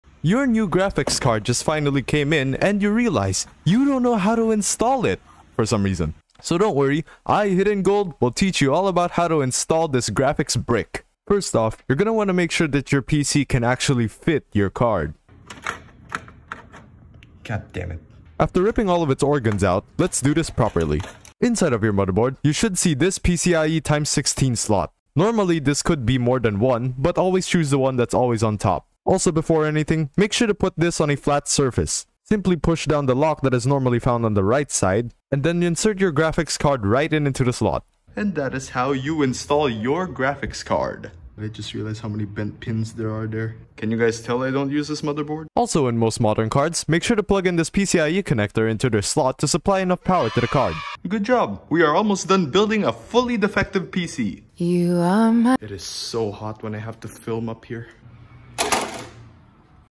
How To Install a Graphics Card!
Simple PC guide about how to install your Graphics Card or GPU!